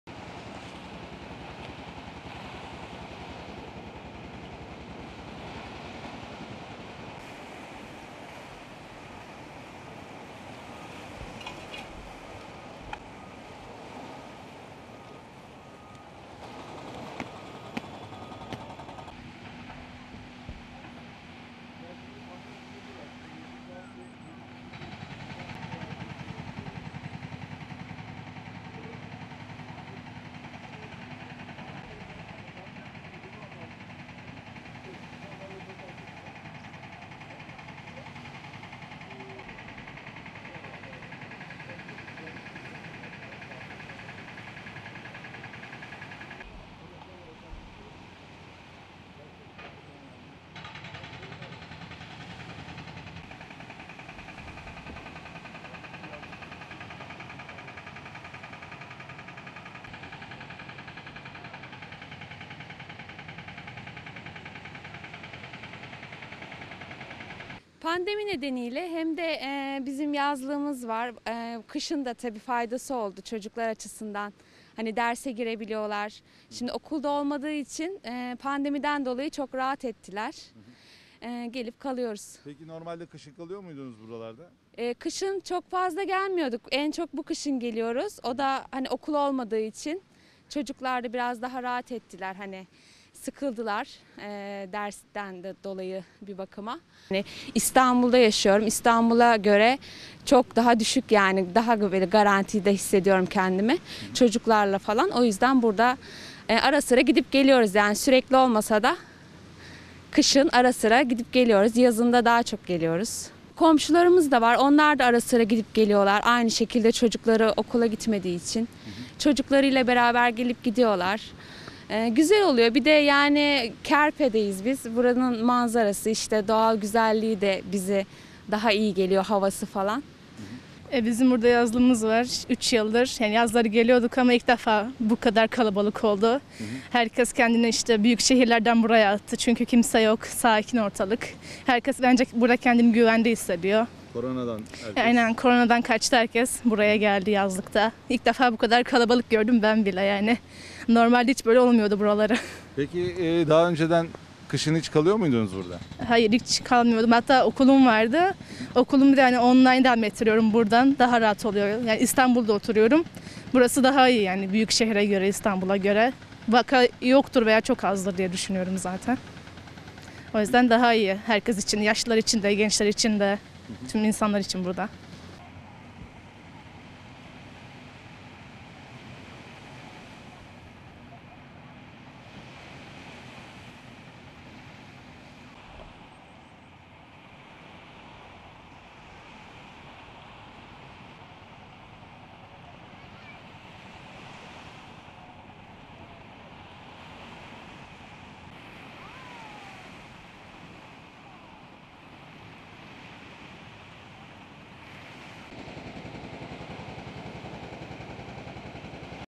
Pandemide yazlık evlerini tercih edenlerin sayısı 5 kat arttı
Kocaeli'nin Karadeniz'e kıyısı bulunan Kandıra ilçesinde yazlıkları bulunan vatandaşlar, pandemi nedeniyle kışı da ilçede geçirmeyi tercih etti. Normalde kış aylarının sakin geçtiği ilçede, bu sene ise nüfus 4- 5 kat arttı. Kerpe Mahalle Muhtarı Ecevit Kaptan, "Kerpe'de önceden kışın yaklaşık 300 kişi kalırdı. Pandemi başlayınca bu sayı 700 çıktı. Şu anda ise yaklaşık bin 500 kişi kalıyor" dedi.
Ergün AYAZ- Alişan KOYUNCU/KANDIRA (Kocaeli), (DHA)